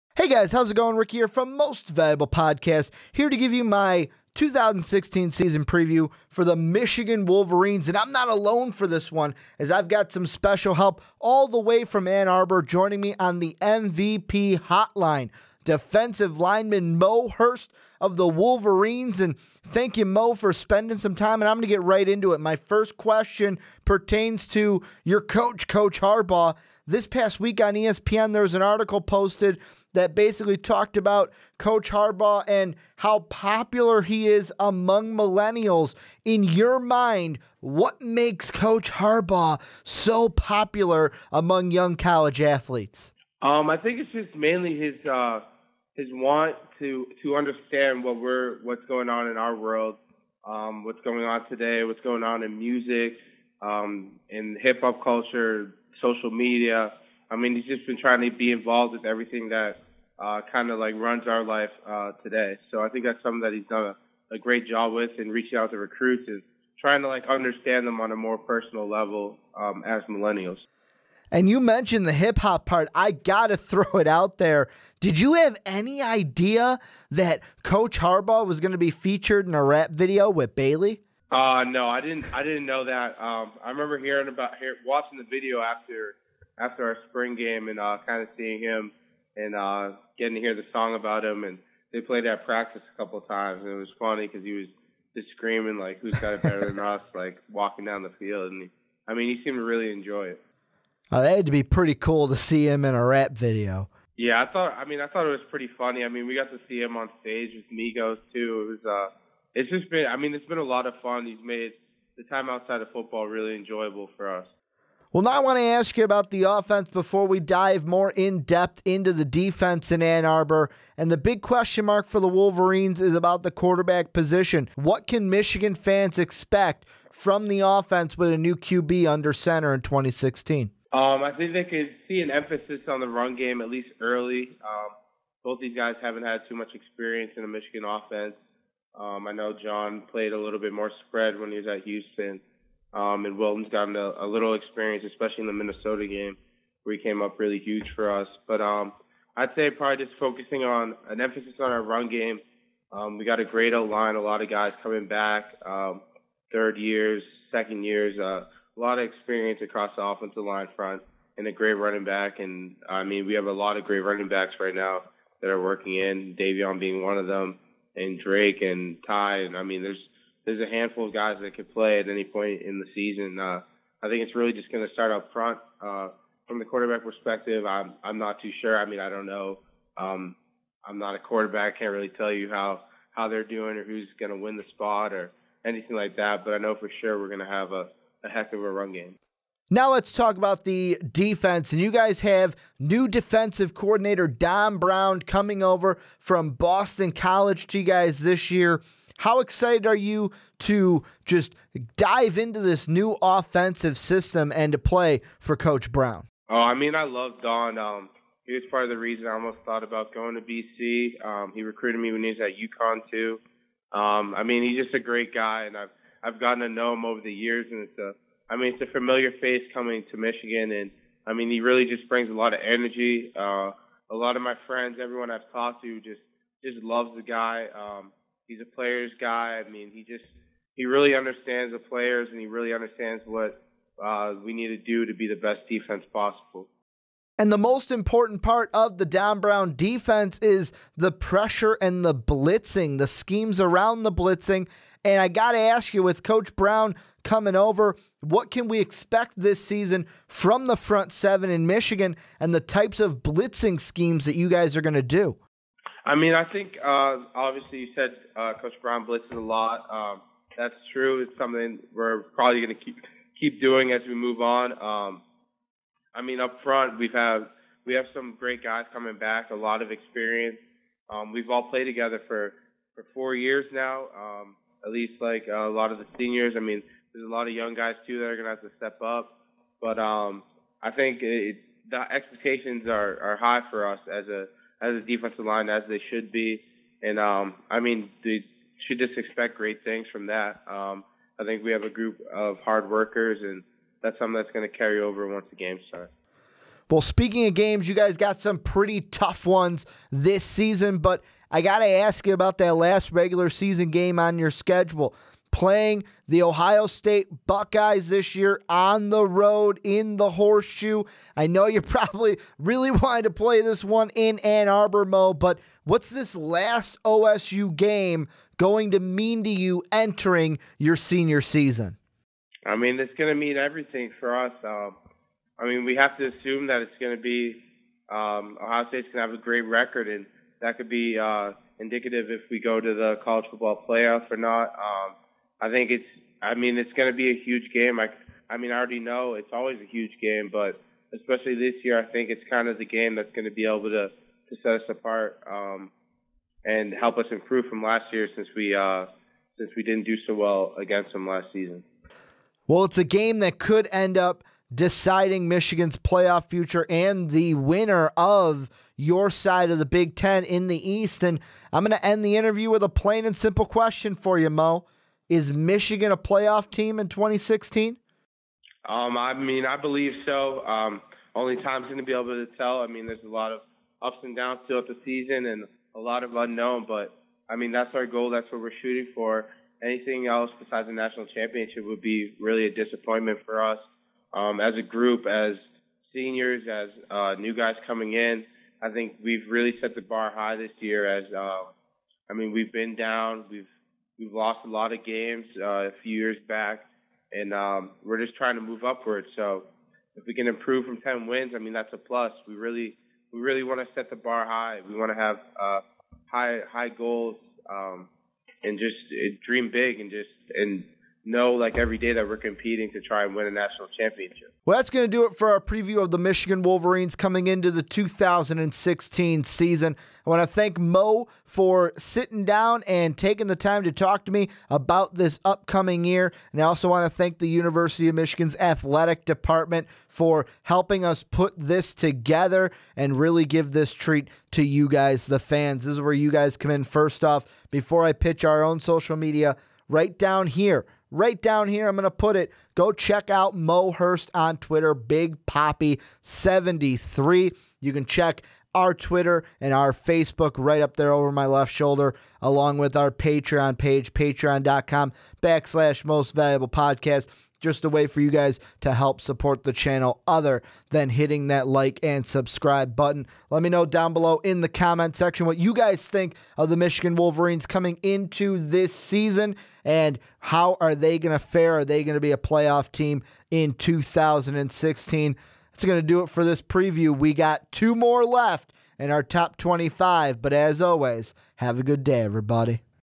Michigan Wolverines 2016 Season Preview (Mo Hurst Interview)
Ricky Widmer sits down with Michigan defensive lineman Mo Hurst to preview the 2016 season for the Michigan Wolverines.

Head shot and Thumbnail Photo Credit: University of Michigan Athletics